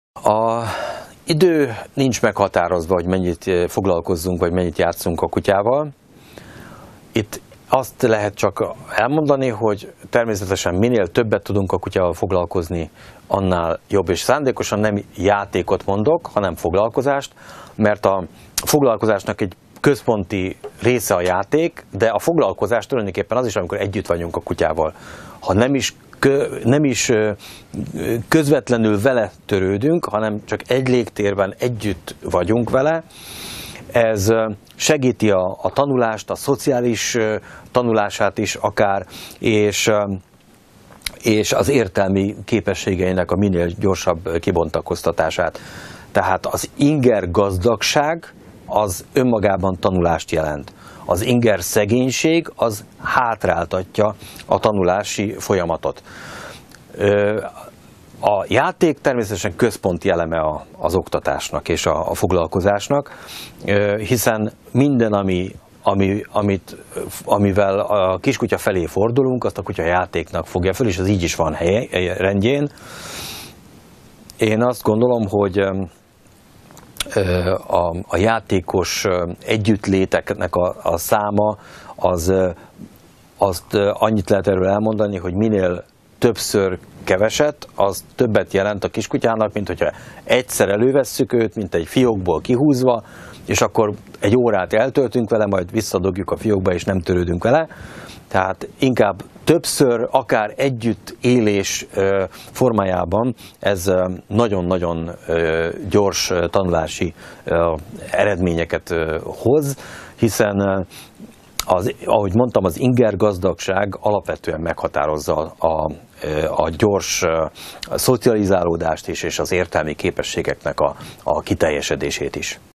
Mennyit kell játszani naponta egy kiskutyával?
Minél többször foglalkozzunk kevesebbet a kiskutyával, vagy naponta csak egyszer, de akkor sokat? Melyik jobb?  Dr. Bánfi András állatorvos erről is beszél a mai Gondos Gazdik videóban.